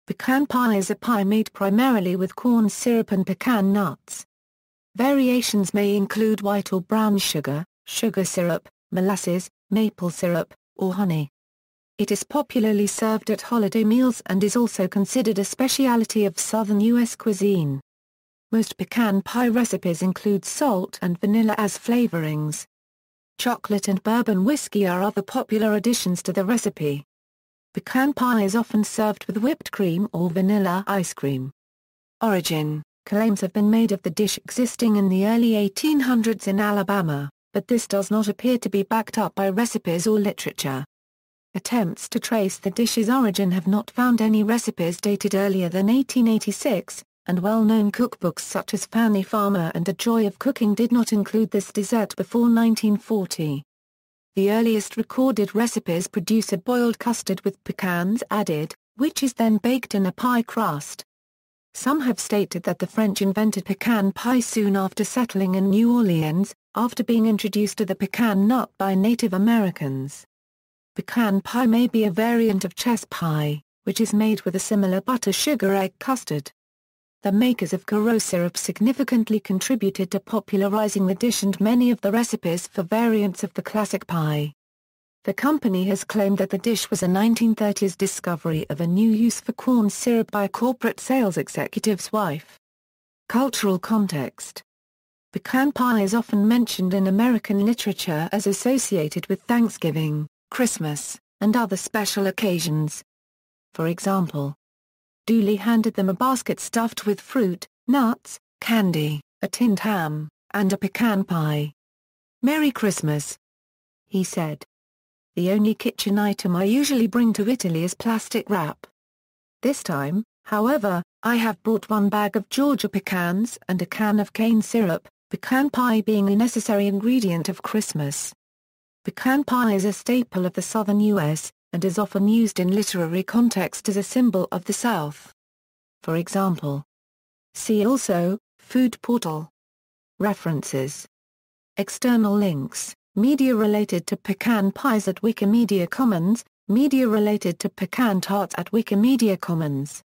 Pecan pie
Pecan pie is a pie made primarily with corn syrup and pecan nuts. Variations may include white or brown sugar, sugar syrup, molasses, maple syrup, or honey. It is popularly served at holiday meals and is also considered a specialty of Southern U.S. cuisine. Most pecan pie recipes include salt and vanilla as flavorings. Chocolate and bourbon whiskey are other popular additions to the recipe. Pecan pie is often served with whipped cream or vanilla ice cream.